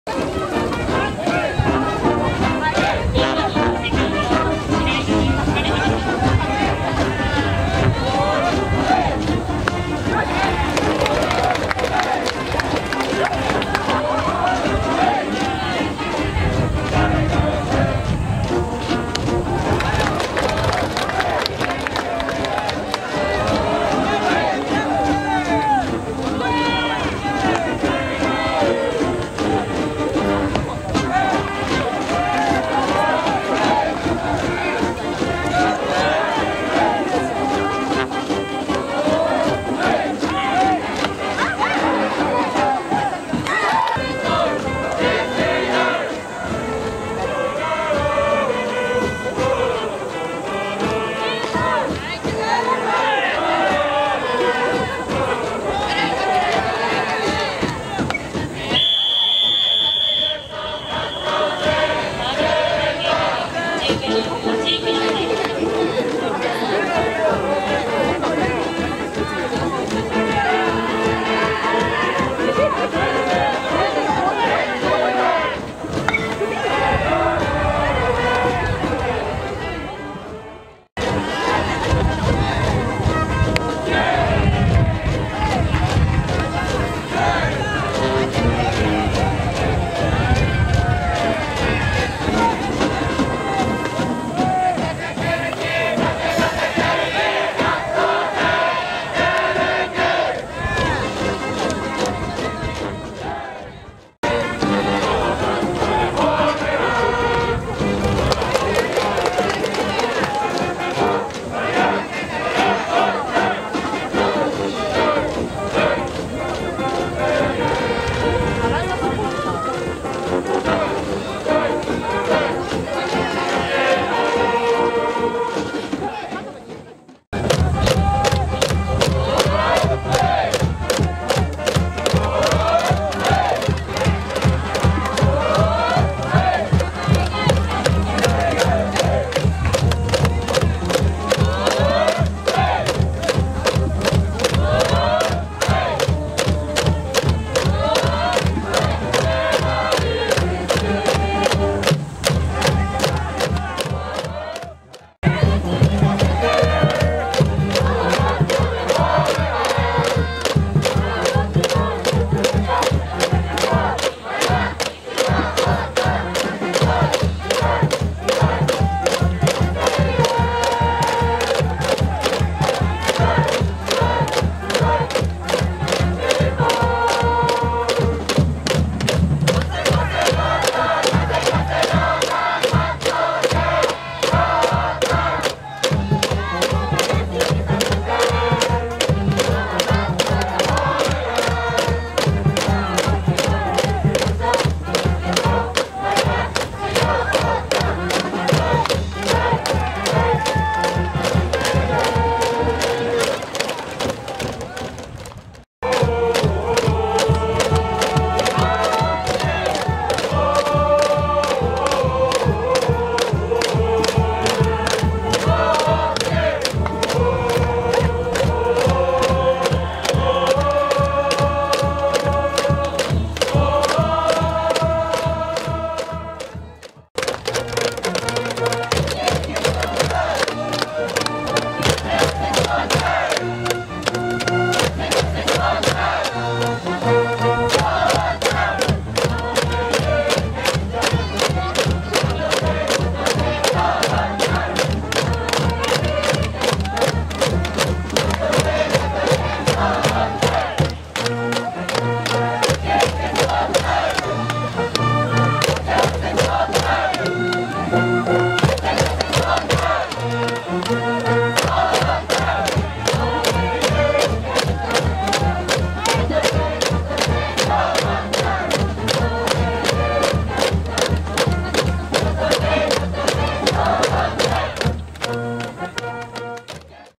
君津商業高校 2017最初で最後の野球応援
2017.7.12 ゼットエーボールパーク（対市立船橋）